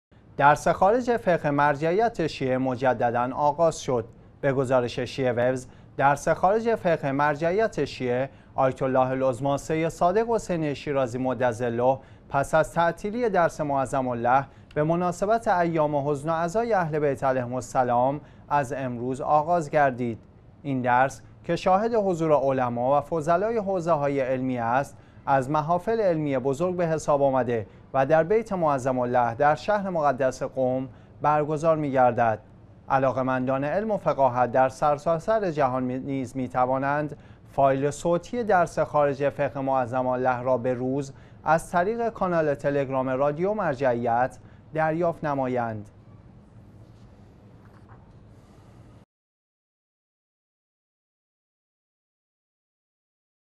از سر گیری درس خارج فقه آیت الله العظمی شیرازی مدظله
درس خارج فقه مرجعیت شیعه مجددا آغاز شد.
به گزارش شیعه ویوز، درس خارج فقه مرجع مرجعیت شیعه آیت الله العظمی سید صادق حسینی شیرازی مدظله پس از تعطیلی درس معظم له به مناسبت ایام حزن و عزای اهل بیت علیهم السلام، از امروز آغاز گردید.
این درس که شاهد حضور علما و فضلای حوزه های علمیه است، از محفل علمی بزرگ به حساب آمده و در بیت معظم له در شهر مقدس قم برگزار می گردد.
علاقه مندان علم و فقاهت در سرتاسر جهان نیز می توانند، فایل صوتی درس خارج فقه معظم له را (به روز) از طریق کانال تلگرام «رادیو مرجعیت» دریافت نمایند.